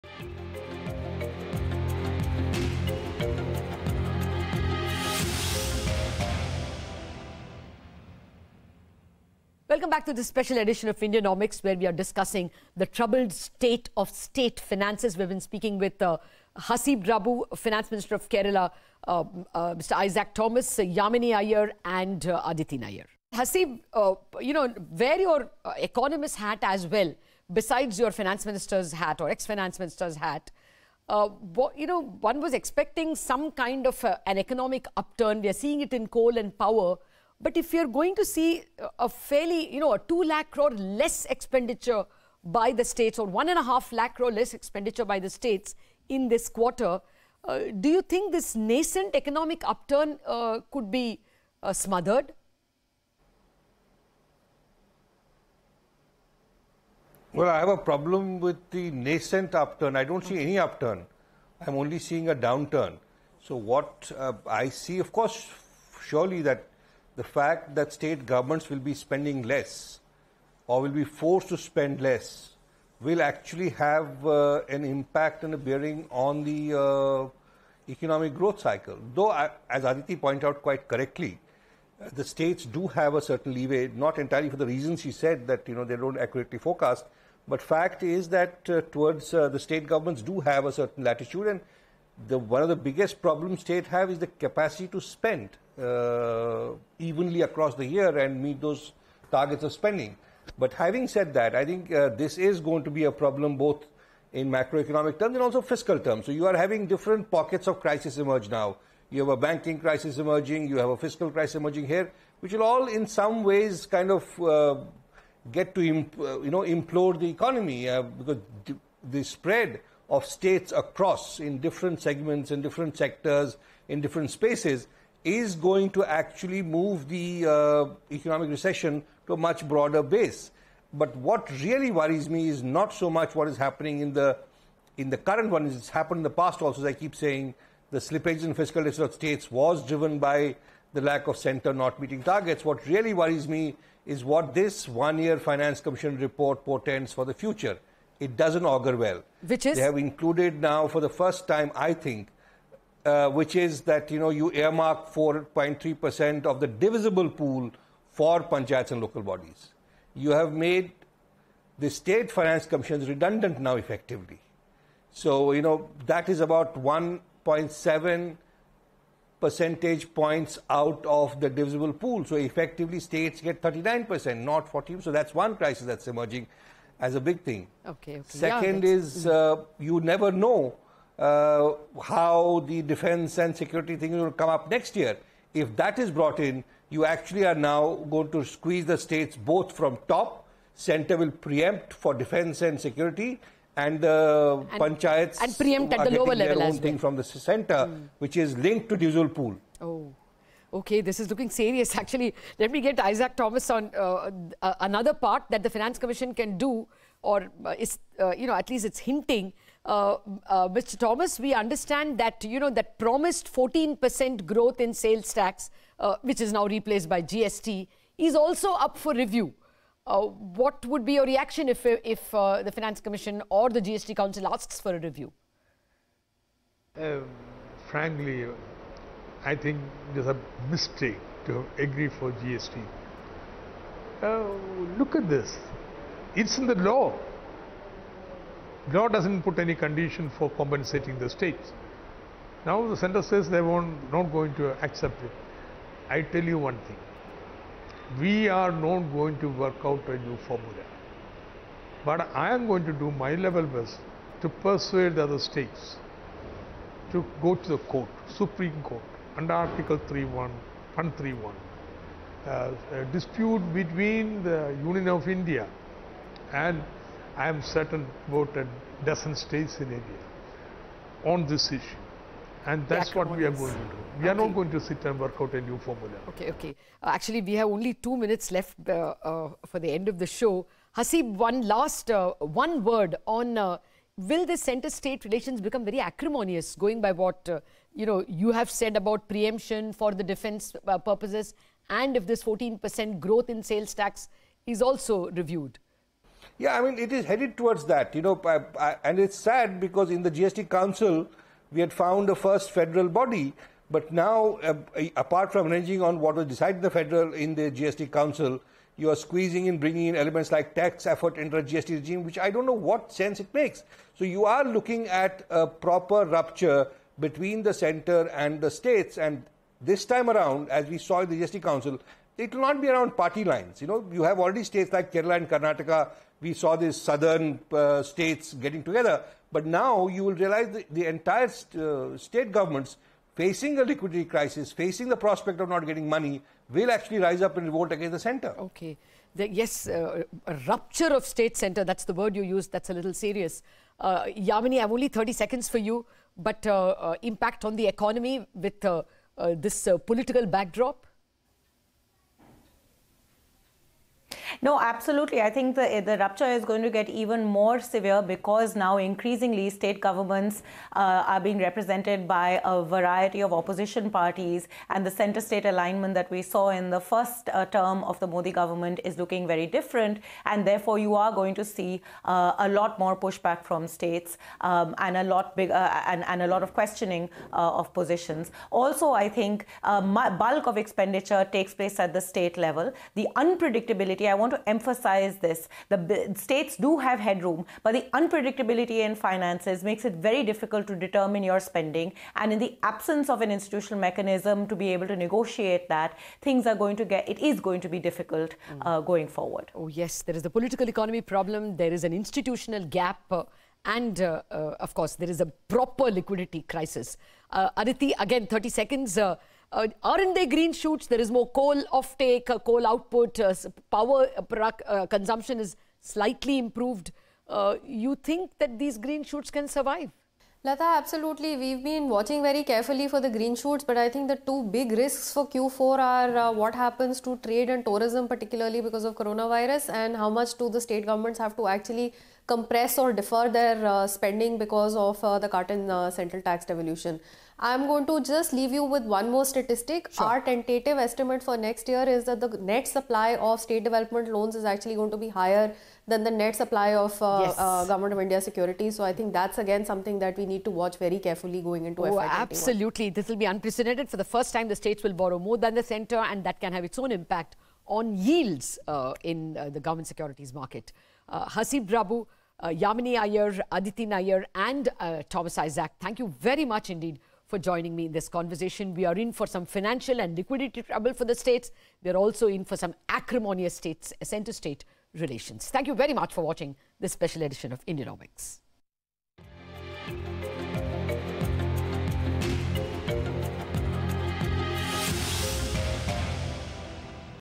Indianomics - State Finances (Part 2)
States finances are under stress as central government delays payments. Centre's share to states on tax revenue also sees a drop. What impact will this have on economic growth? Latha Venkatesh discusses this with Thomas Isaac, Finance Minister of Kerala; Haseeb Drabu, Economist & Former Finance Minister of J&K; Yamini Aiyar, President of the Centre for Policy Research; & Aditi Nayar, Principal Economist at ICRA.